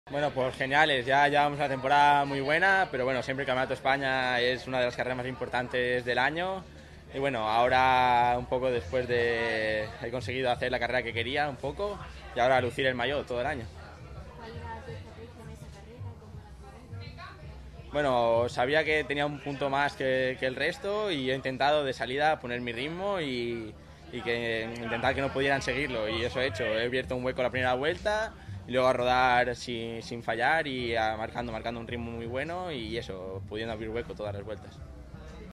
CAMPEONATO ESPAÑA CX 2019 / FELIPE ORTS CAMPEÓN ÉLITE
CECXPontevedra19 | Felipe Orts el nuevo campeón en la categoría Élite nos cuenta como ha vivido su victoria.

"He marcado un buen ritmo desde la primera vuelta, he conseguido hacer la carrera que quería y ahora toca lucir el maillot todo el año"